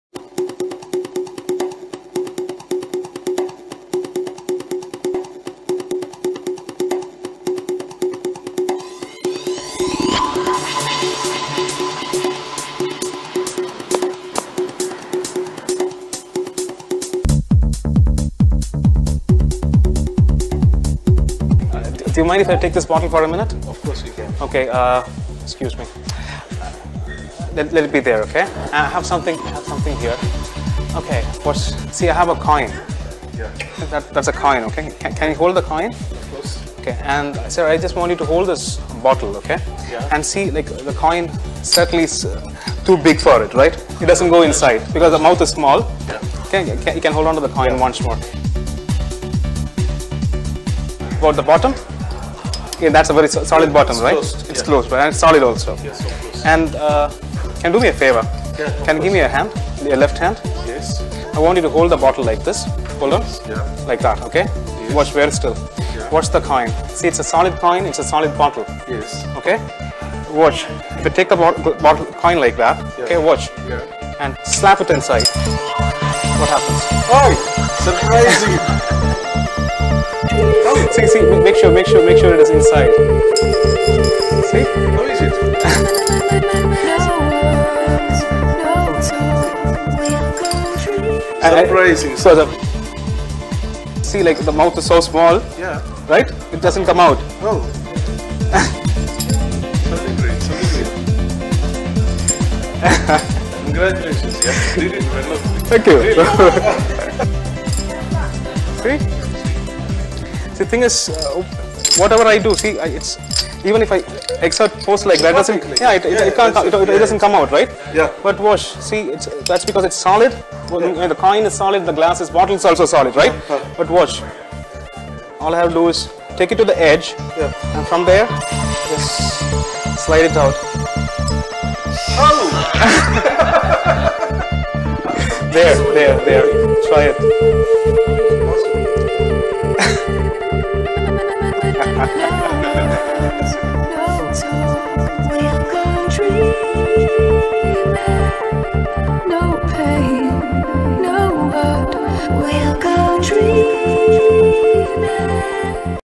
Magician does the IMPOSSIBLE with a coin and bottle! Raja Moorthy - 2007
This was shot in Trivandrum during 2007 as a part of a television series.